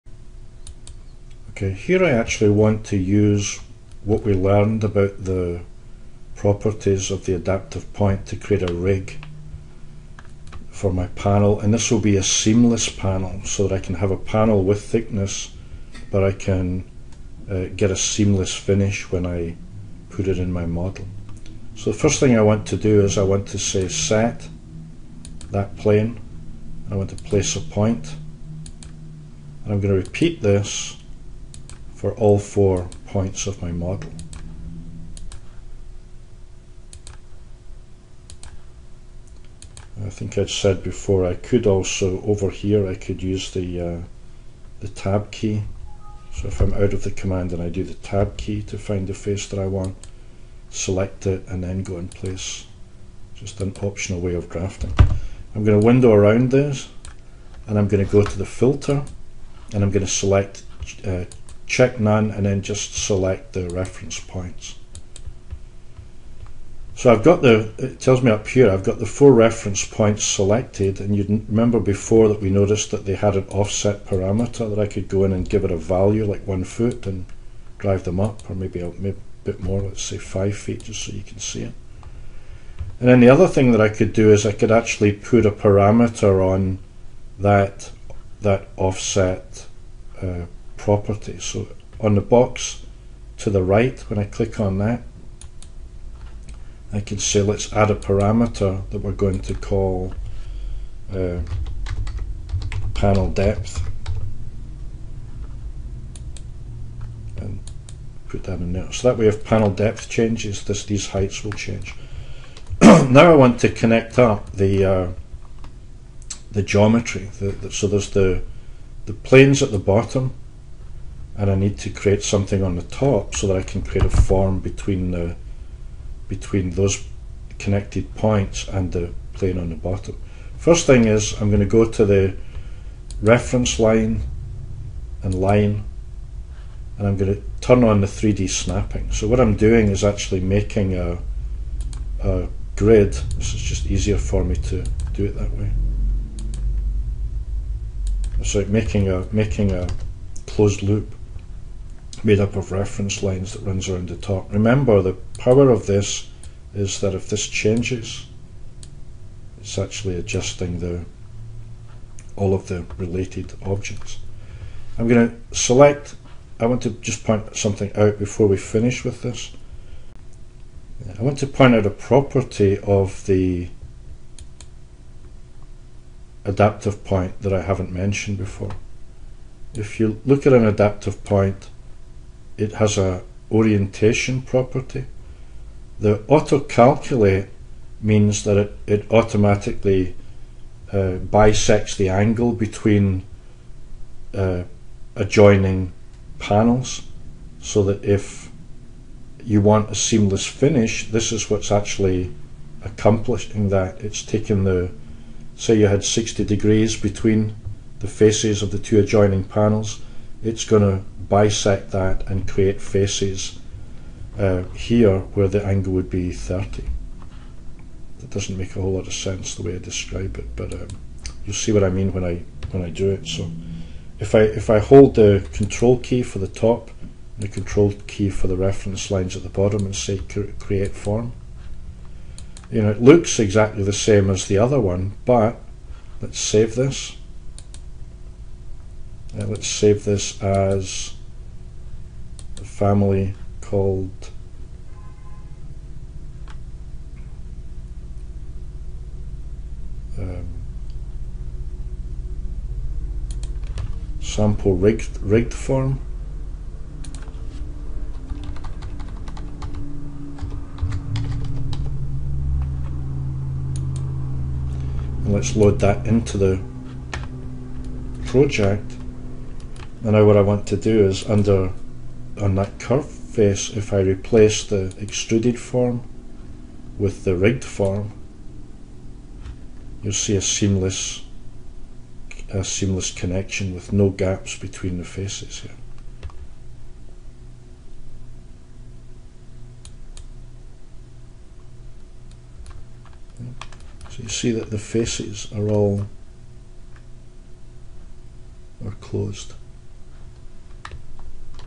pad02 Creating a panel based on a rig that gives blended surfaces.
Creating a curtain wall pattern, based panel that creates a blended surface (seamless) on a curved complex form.